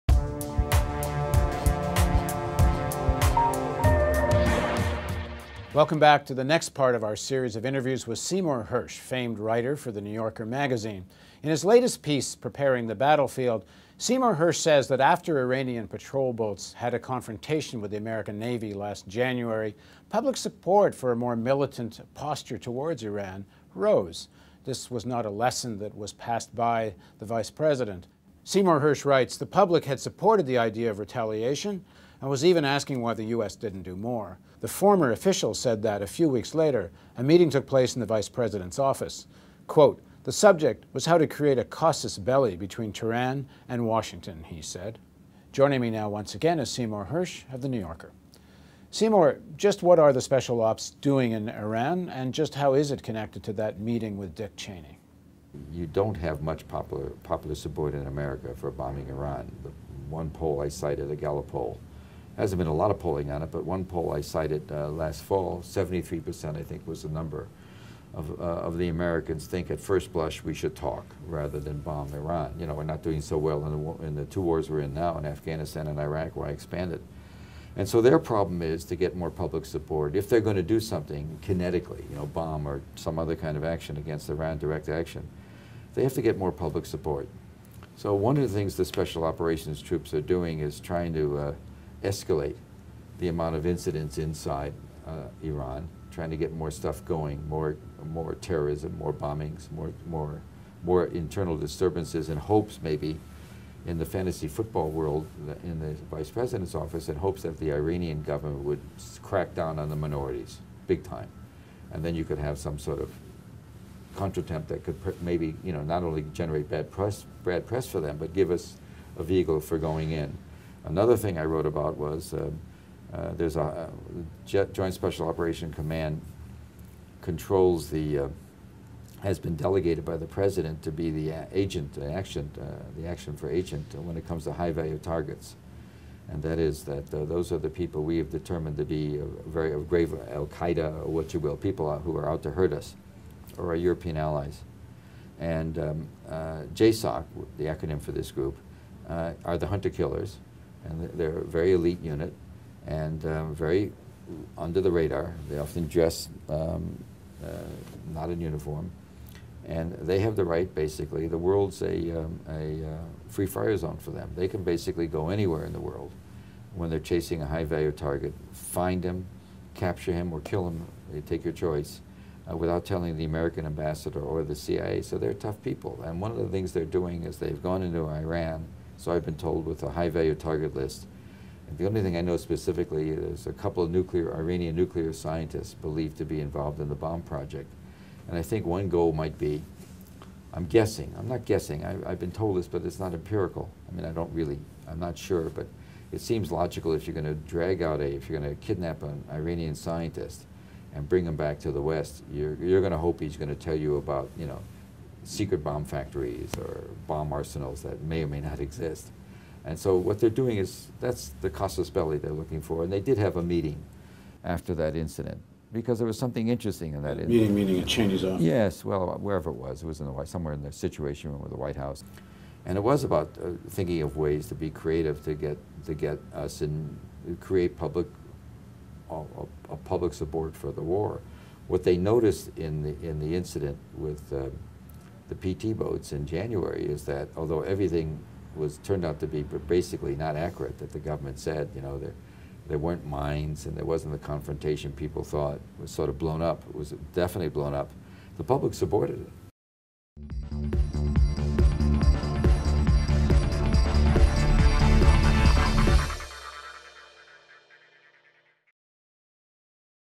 Cheney and manufacturing consent on Iran
Seymour Hersh: The secret war in Iran (2 of 3)